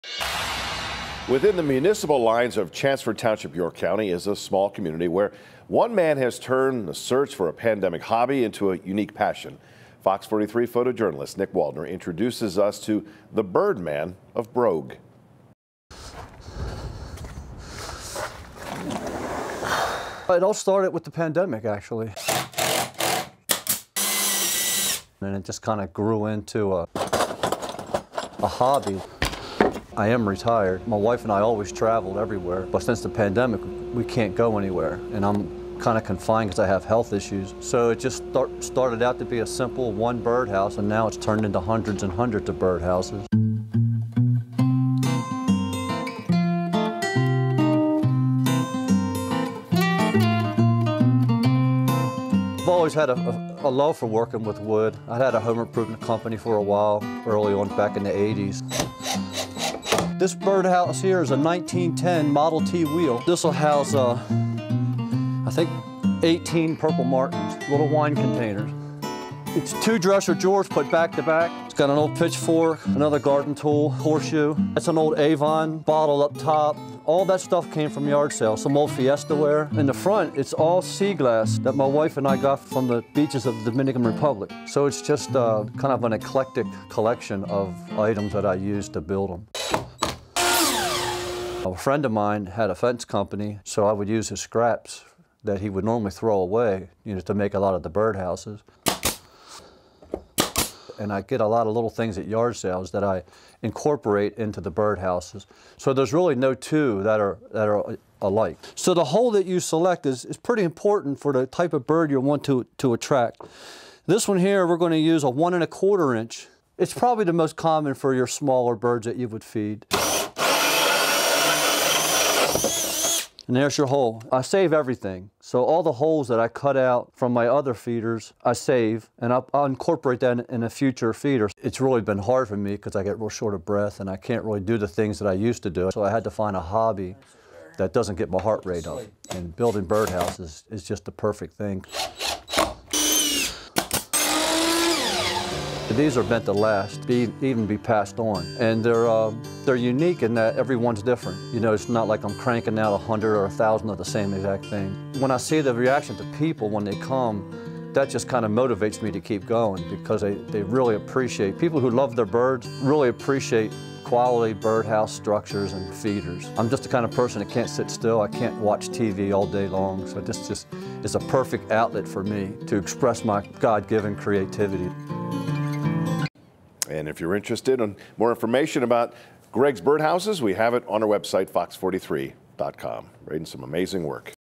Birdman of Brogue | York County man's pandemic hobby turns into creation of hundreds of birdhouses
One man's hobby of building birdhouses has taken off amid the COVID-19 pandemic.

Greg "The Birdman of Brogue" Germroth says that he and his wife used to travel but have not been able to since the pandemic began due to his health issues.